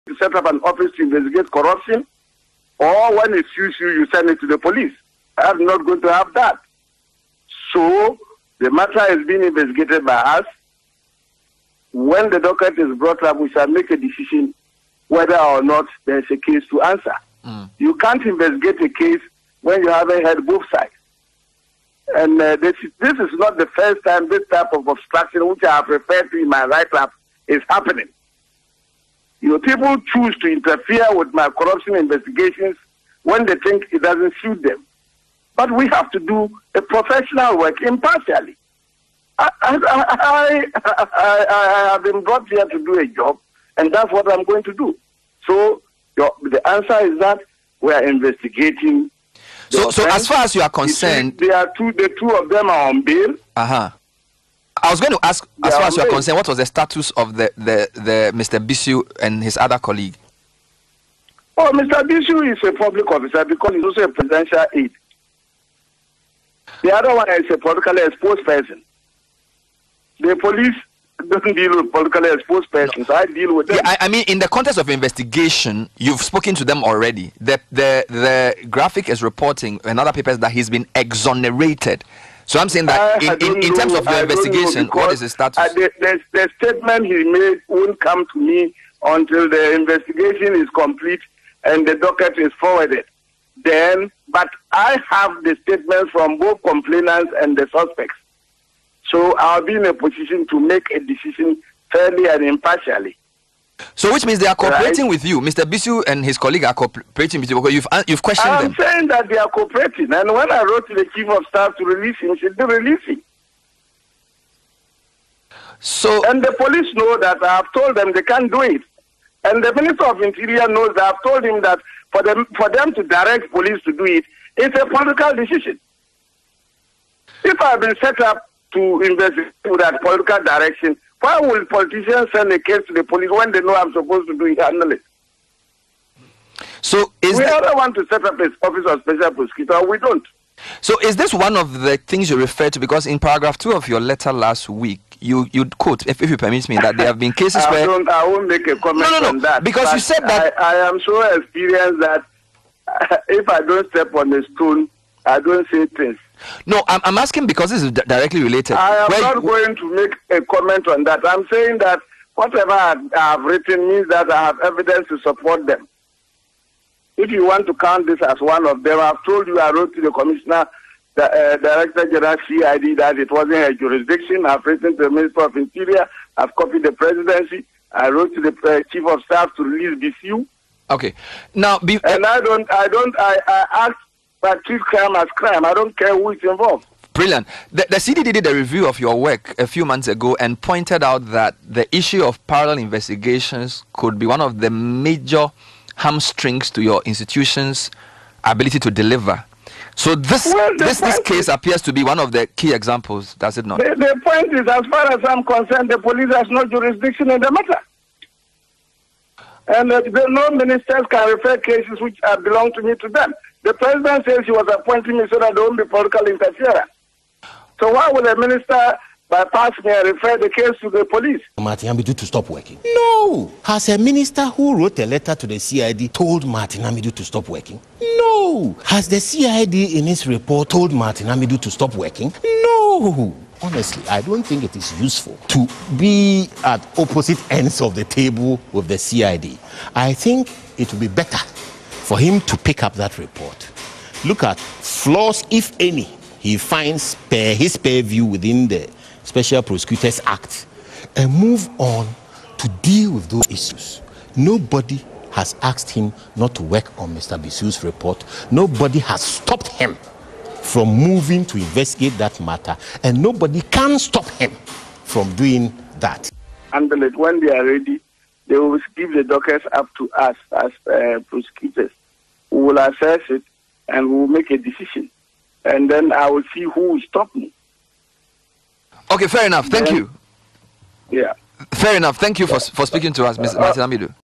Amidu complaining too much, nobody is stopping his probe – Buabeng Asamoah
The Communications Director for the governing New Patriotic Party (NPP), Yaw Buaben Asamoah is urging the Special Prosecutor, Martin Amidu to stop complaining about interference in his work and continue to serve as expected of him.

He said so long as no one is preventing the Special Prosecutor from carrying his work, he must stay focused and discharge his duties professionally.

The Adentan legislator made the comments on the back of complaints in a recent article by Martin Amidu that some heads of state institutions were interfering or not cooperating with him.

Martin Amidu revealed subsequently that despite his calls on the Interior Ministry to stop the CID from continuing investigations into the allegation of corruption brought against a presidential staffer, Charles Bissue, in a matter the Special Prosecutor believes falls within his jurisdiction, the Interior Minister allowed the Police CID to complete investigations which allegedly exonerated Mr. Bissue.

But Mr. Buaben said the CID’s work in no way interfered with or affected investigations by the Office of the Special Prosecutor.

Mr. Buaben Asamoah said Mr. Amidu can pick up the CID report and pursue the case if he feels dissatisfied.

“Why is he complaining all the time. Let him do his work. Has the president told Martin Amidu to stop work, no. Has a minister who wrote a letter to the CID told Martin Amidu to stop work, no. Has the CID in its report to Martin Amidu told him to stop work? No. I don’t think it will be useful to be at opposite ends of the table with the CID. I think it will be better for him to pick up that report, look at flaws if any he finds per his view within the Special Prosecutor’s act and move on to deal with those issues. Nobody has stopped him or can stop him from moving to investigative that matter,” Yaw Buabeng Asamoah said.

An investigative piece by Anas Aremeyaw Anas captured the then Secretary to the anti-galamsey taskforce allegedly taking a bribe to allow mining without due process.Former Central Regional Chairman of the opposition National Democratic Congress (NDC) says he has had enough of the ranting of Special Prosecutor, Martin Amidu.

According to Bernard Allotey Jacobs, the continuous complaints by Mr Amidu regarding investigations on the ‘Galamsey Fraud’ expose’ by Anas Aremeyaw Anas “expose his weakness.”

His comment followed another epistle from the Special Prosecutor. This time his complaint was that heads of State institutions were deliberately impeding the mandate of his office.